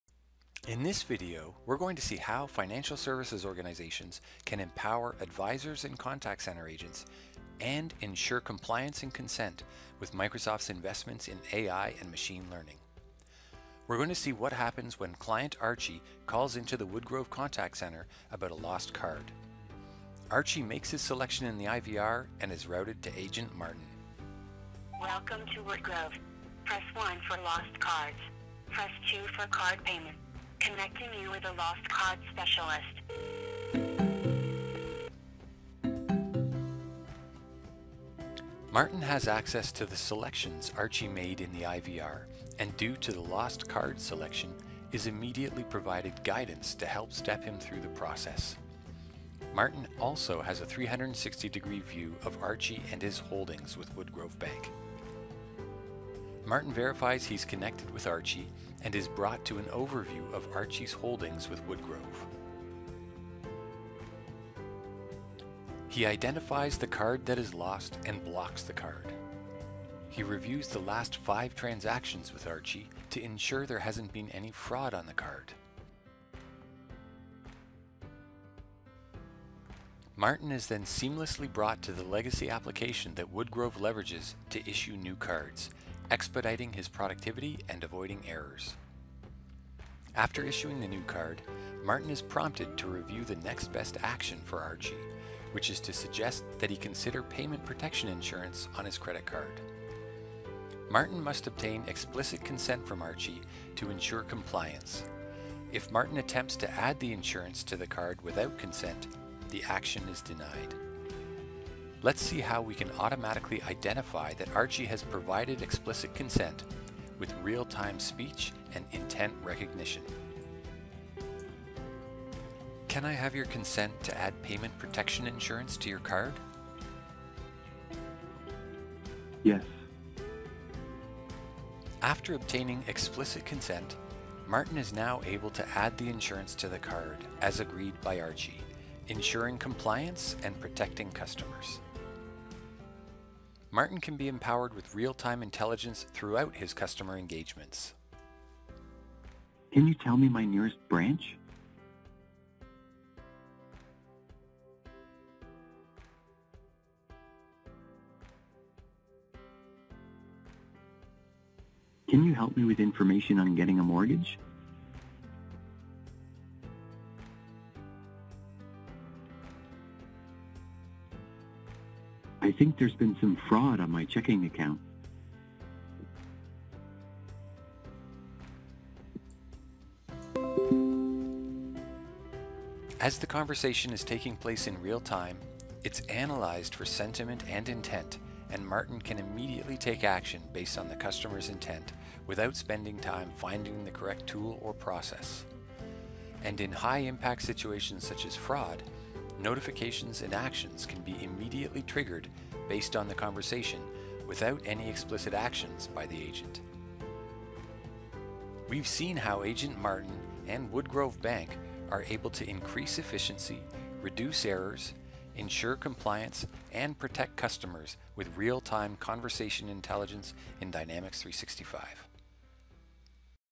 AI for Compliance and Empowerment in Financial Services Contact Centers
In this video, we see how financial services organizations can empower advisors and contact center agents, and ensure compliance and consent, with Microsoft's investments in AI and Machine Learning.

Jump to Segments:
- Real-time Voice-based Consent: 1m40s
- Empowering Agents with Real-time Intelligence: 2m22s